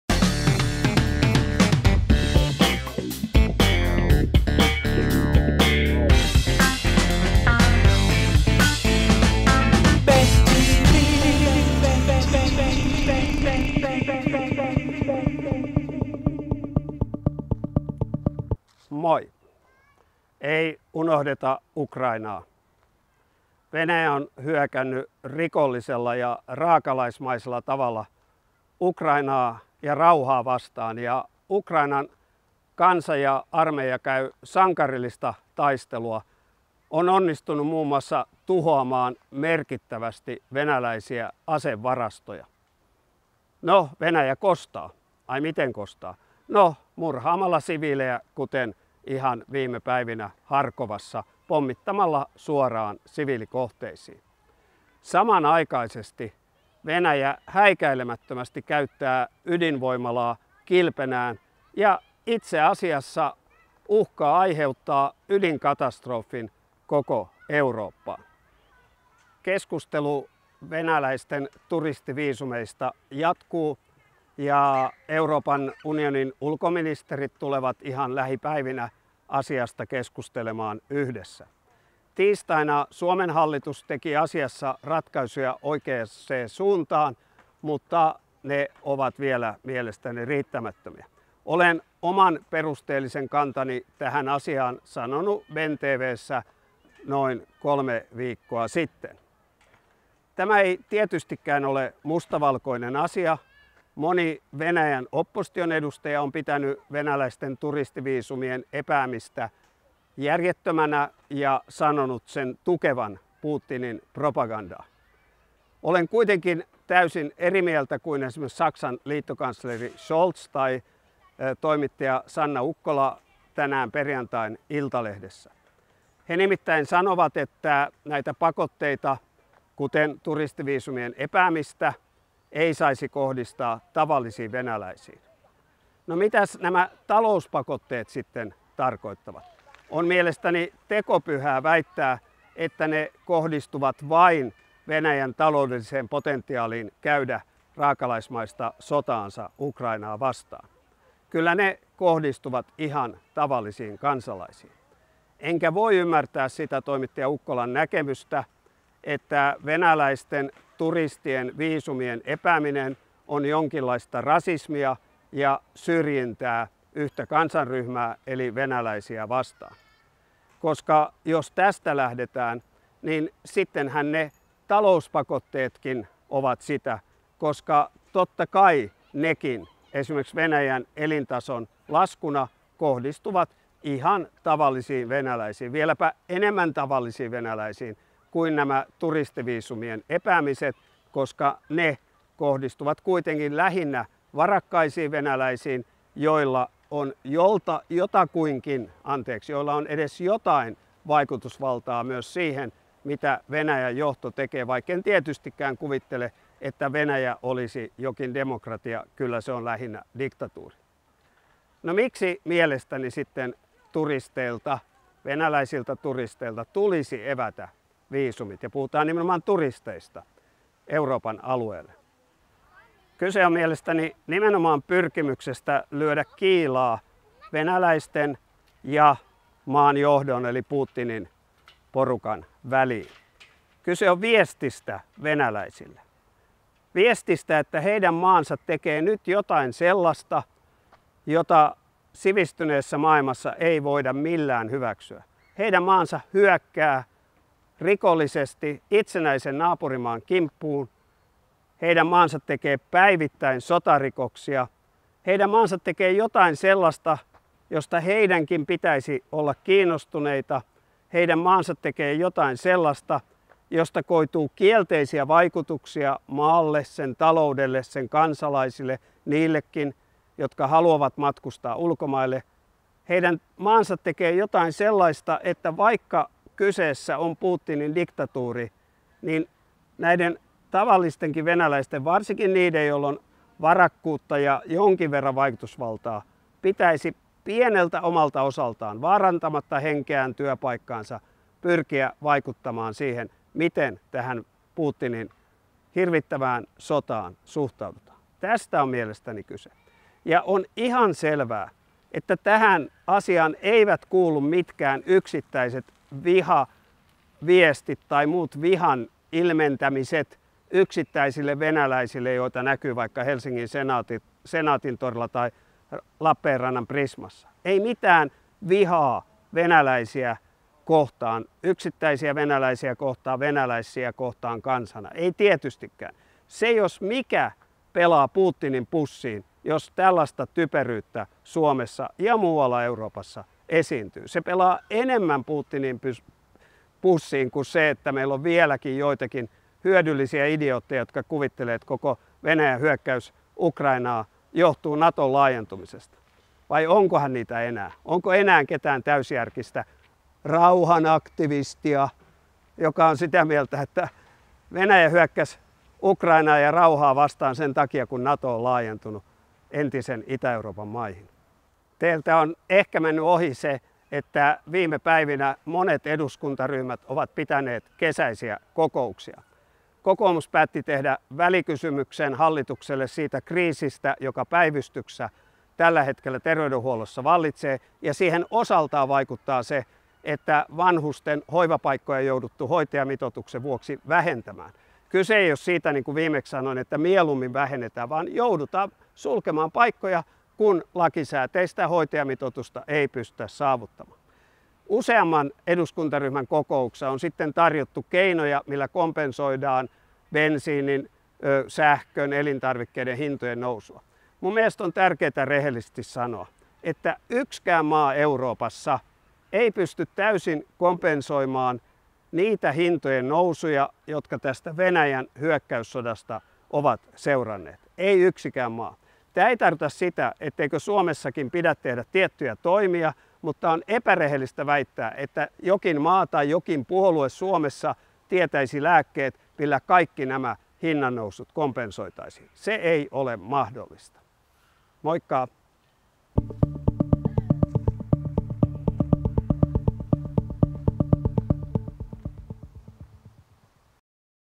BenTV: Venäläisviha pelaa Putinin pussiin
Yksittäisiin venäläisiin Suomessa kohdistuva rasismi on erittäin haitallista.